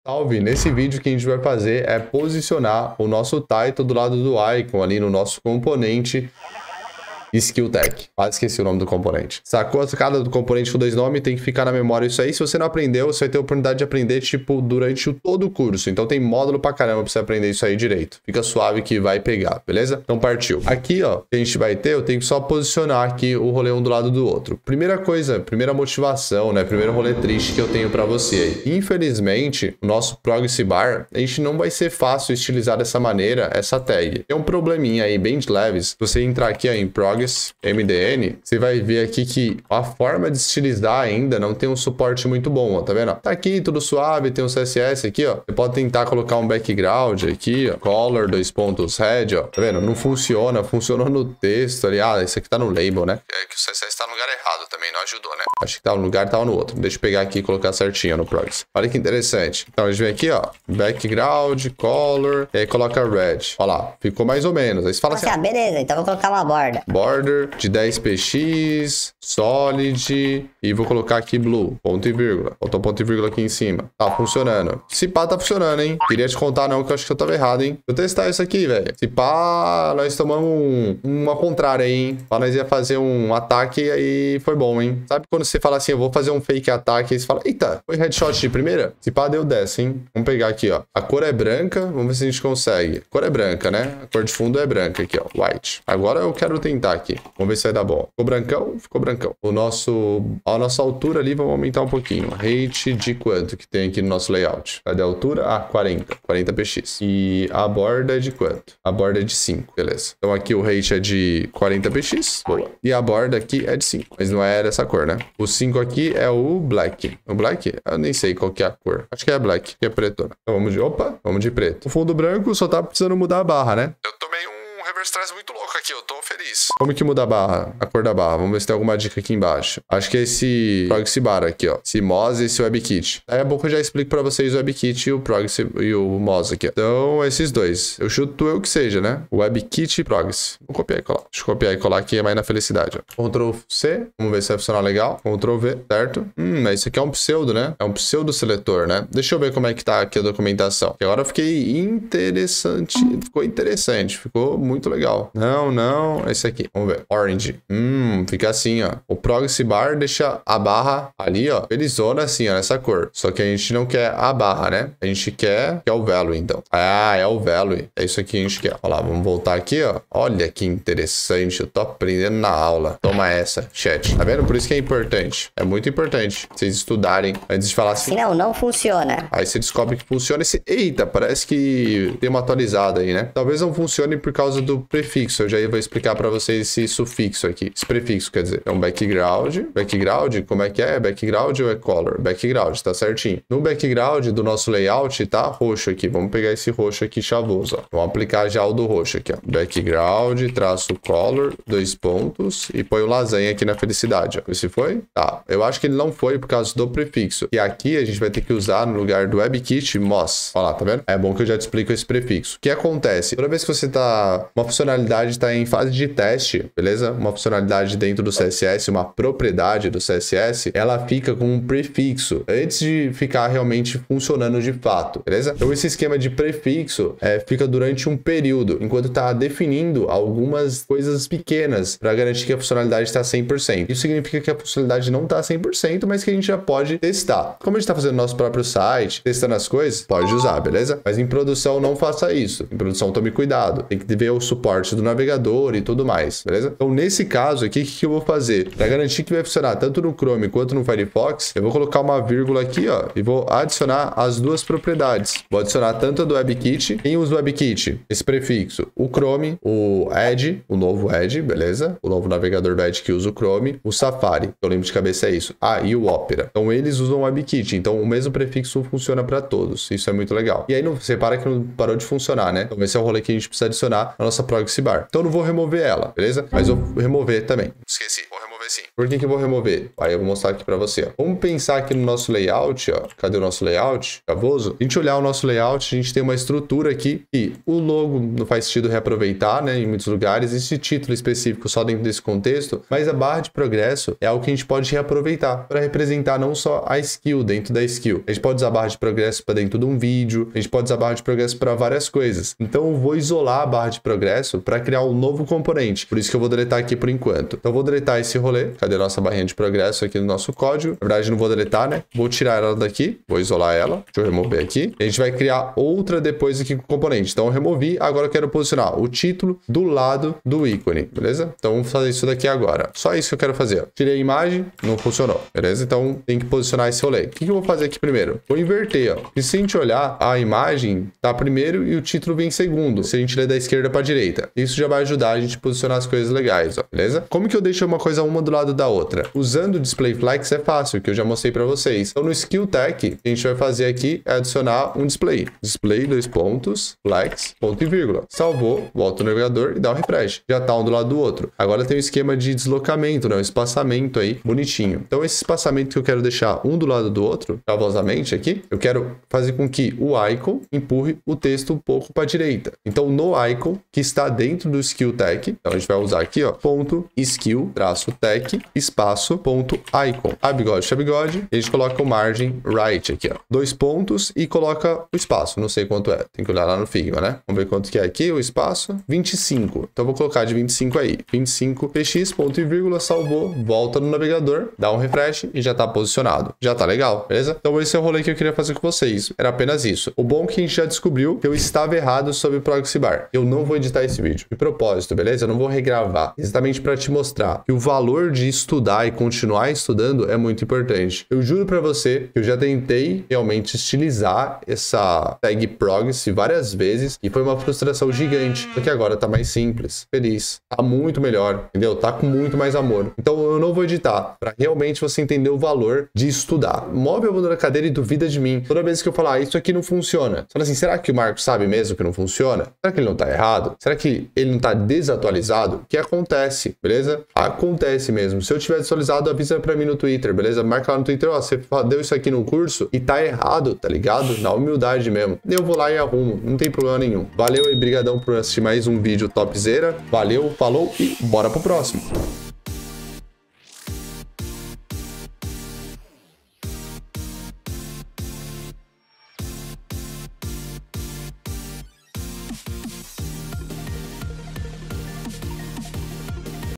Como posicionar ícone e título | Mód04 - Aula 07
Sem corte, mostrando pra vocês na prática o valor de estudar e continuar estudando!
Na aula vemos juntos como posicionar o title do lado do ícone, mas antes falamos um pouco mais sobre a tag progress.